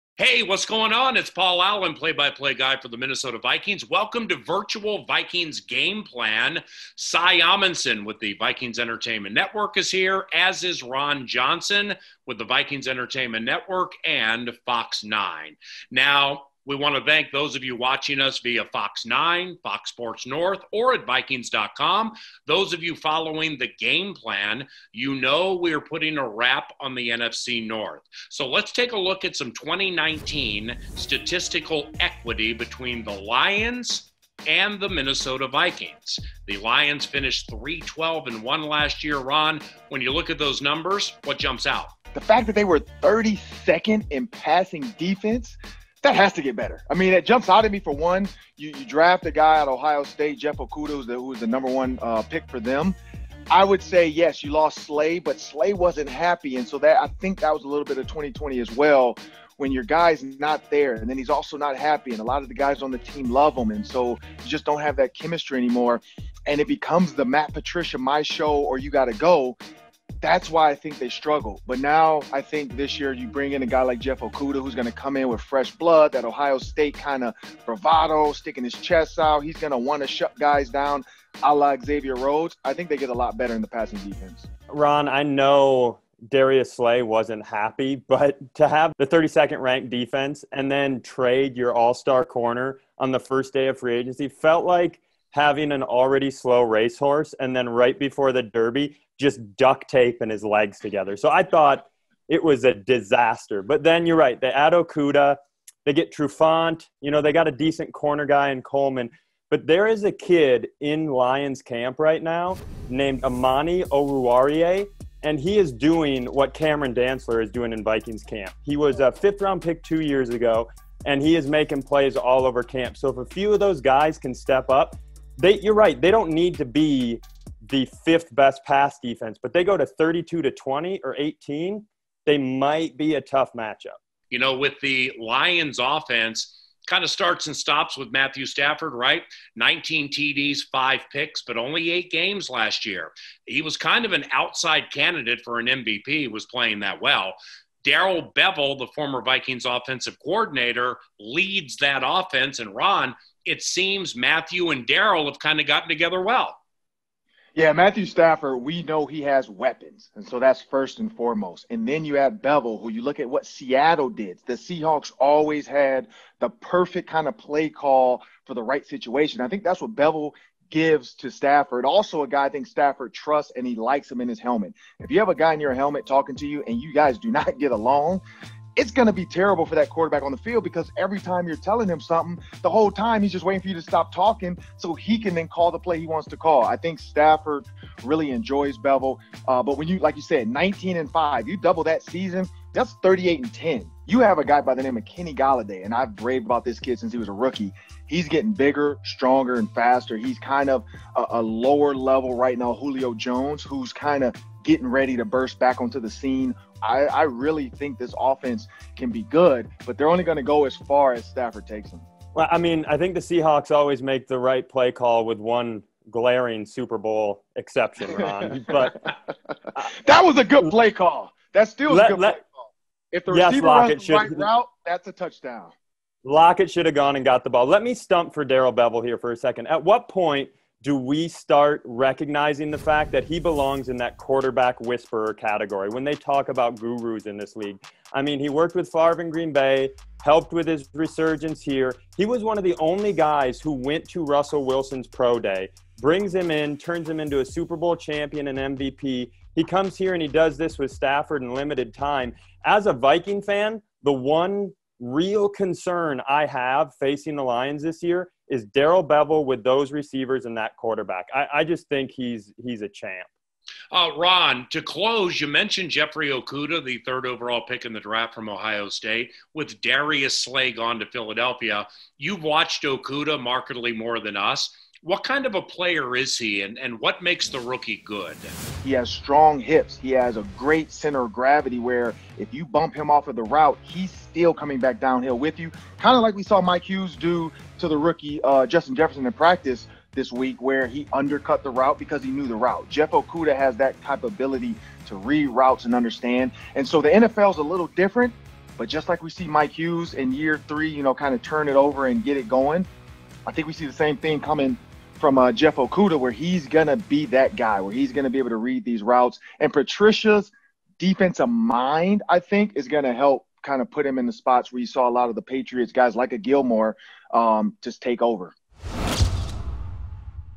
NFC North Season Preview: Detroit Lions | Minnesota Vikings
Paul Allen is joined by Ron Johnson and Cy Amundson to take a look at where the Detroit Lions stand heading into the 2020 NFL season for the Minnesota Vikings.